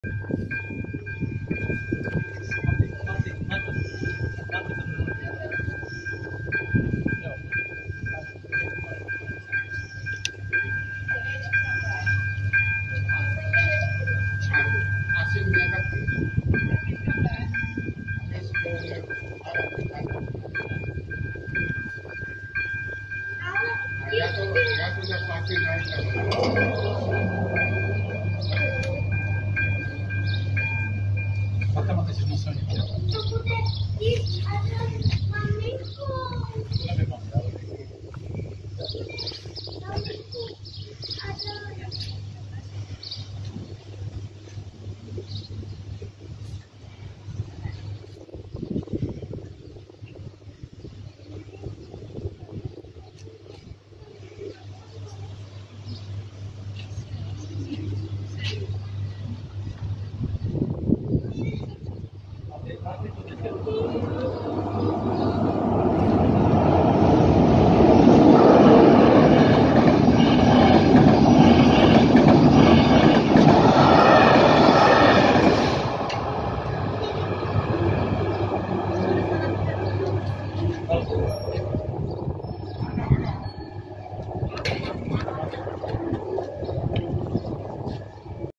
Železniční přejezd P 247 Zadní Třebaň 16.7.2023
Nové výstražníky na přejezdu P247 zadní třebaň (dříve tam bylo ažd 71)